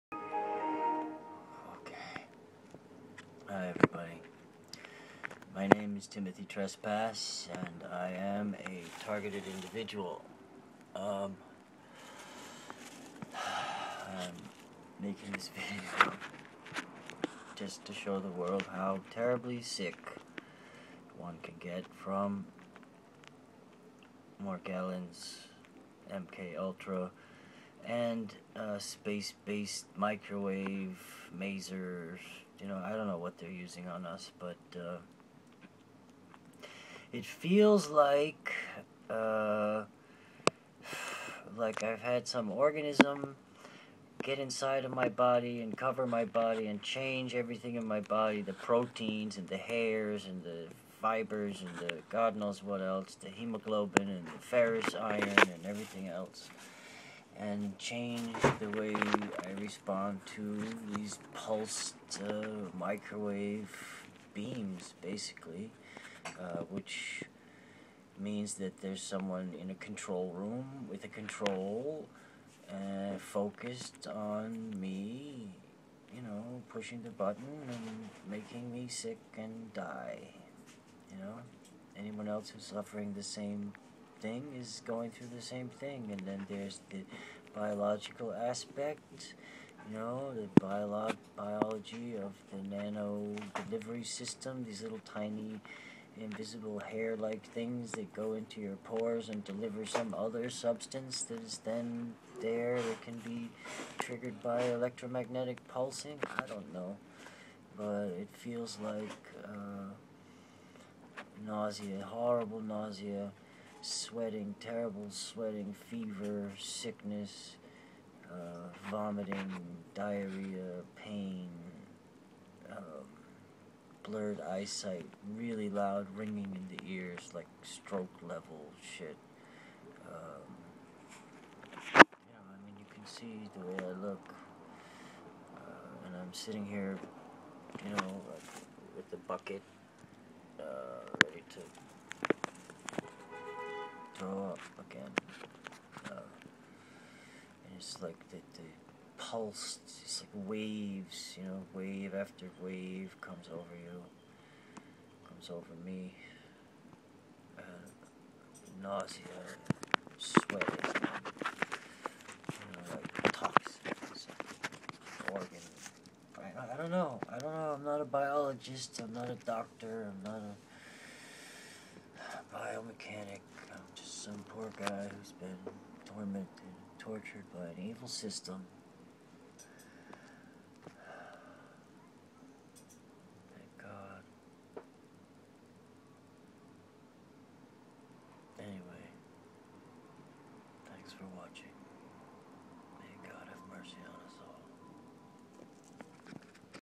Timothytrespas: Microwave Murder of innocents by Evil men! Why is this allowed?
Timothytrespas:microwave murder of innocents by evil men. Watch me sick and dying. Posting these videos sucks because I hate to be so sick. I am clearly being targeted by technology run by evil men.What more can I say.
Morgellons microwave nanotechnology causing  Evil murder sickness caused by evil men.
How many will die before this great deception is exposed?
 Are we simply going to allow these evil ones to take control of everything?
 Will anyone do anything?
We are in a crazy time and it is all being done on purpose to reach a goal.
 Do you want to live in a world where evil men control everything?
Your thoughts and emotions are your last freedom and that has been removed.
We have never been free and it seems doubtful humanity will ever be free.
may GOD have mercy on us all.